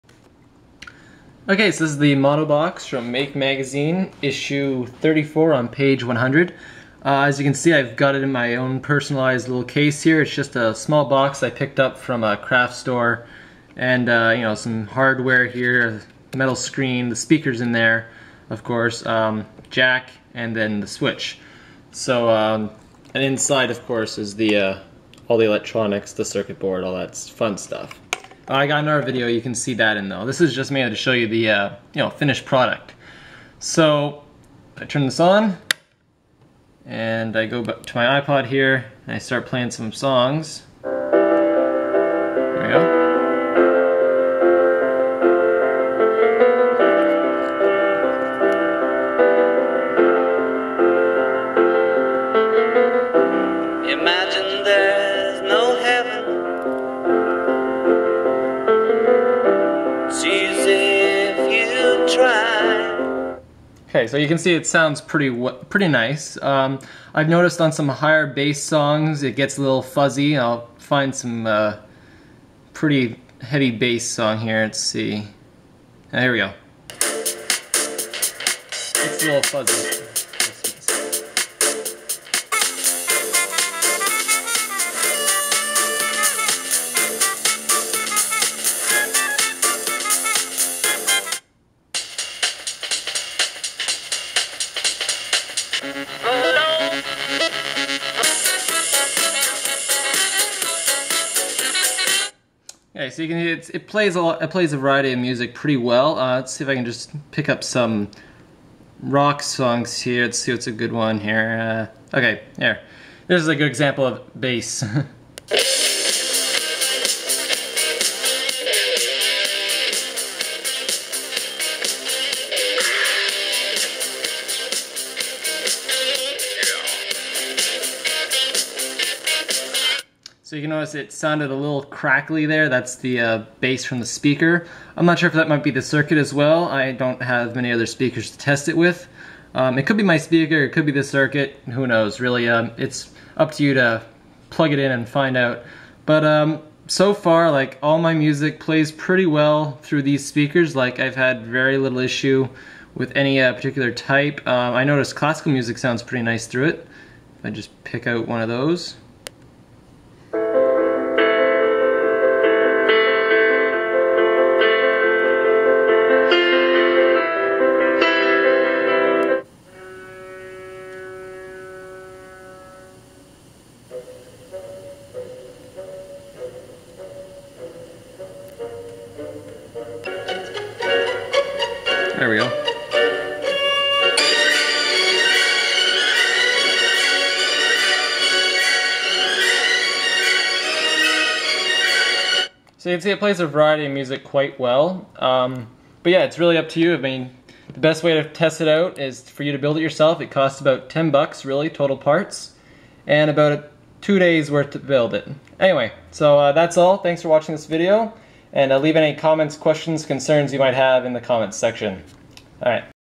The Monobox Amp
A project from make magazine issue 34, on page 100. It's basically a simple amp circuit.